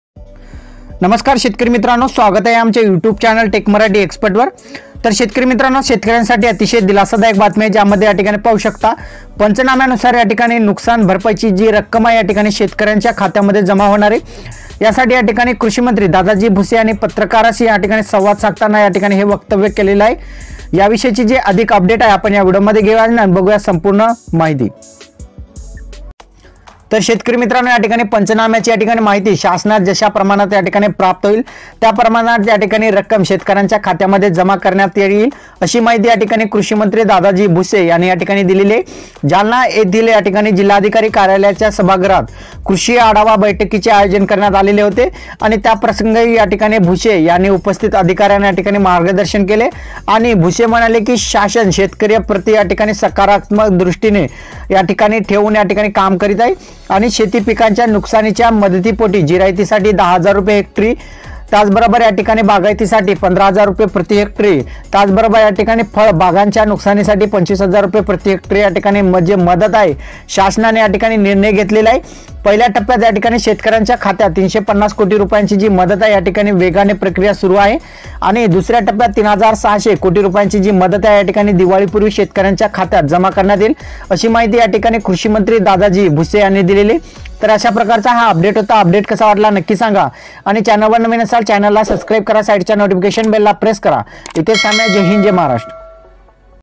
पंचनाम्यानुसार भरपाई ची रक्कम जमा होणार कृषि मंत्री दादाजी भुसे | नुकसान भरपाई 2021
video chapter
00:00 - introduction
00:30 - नुकसान भरपाई यादी